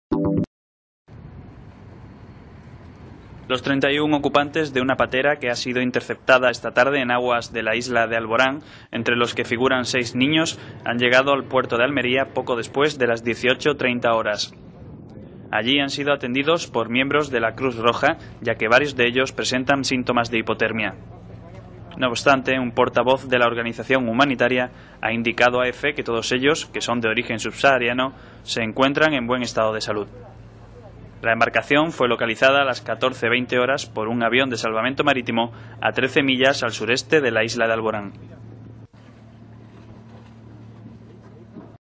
Rescate en alta mar
Almería, 29 oct (EFE).- (Imágenes: Carlos Barba). Los 31 ocupantes de una patera que ha sido interceptada esta tarde en aguas de la isla de Alborán, entre los que figuran seis niños, han arribado al puerto de Almería pasadas las 18:30 horas.
Allí fueron atendidos por miembros de la Cruz Roja ya que varios de ellos presentan síntomas de hipotermia.
No obstante, un portavoz de la organización humanitaria ha indicado a Efe que todos ellos, que son de origen subsahariano, se encuentran en buen estado de salud.
Salvamento Marítimo recibió a las 12:15 horas la llamada de una cooperante alertando de que una patera con quince hombres, diez mujeres y seis niños a bordo había partido del cabo de Tres Forcas la pasada madrugada, por lo que el centro de Coordinación de Almería activó su búsqueda.
La embarcación fue localizada a las 14:20 horas por el avión Sasemar 101, a trece millas al sureste de la isla de Alborán, mientras que la embarcación Guardamar Calíope se ha encargado del traslado de sus ocupantes hasta Almería. EFE.
WWW:EFE:COM. Palabras clave: efe,sucesos,inmigrantes,patera,almeria